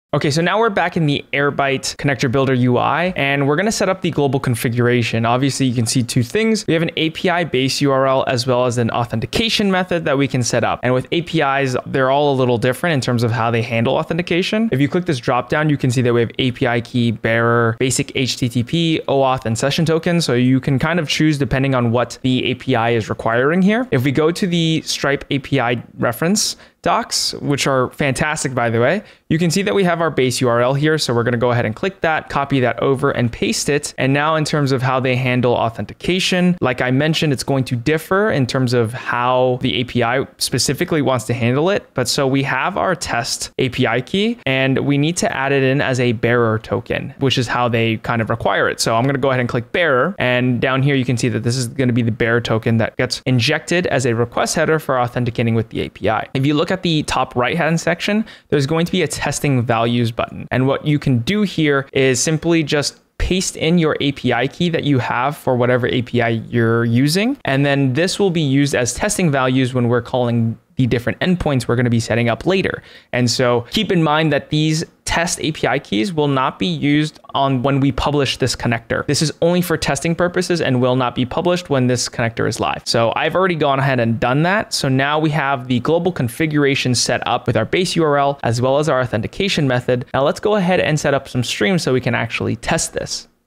Airbyte Connector Builder Tutorial #3 - Global Configuration
In Airbyte Connector Builder Tutorial #3, we delve into Global Configuration using the Airbyte Connector Builder. This session covers the essential settings and configurations needed for your Airbyte Custom Connector. Learn how to effectively use the Airbyte CDK to ensure your connector is properly set up in our Airbyte tutorials series.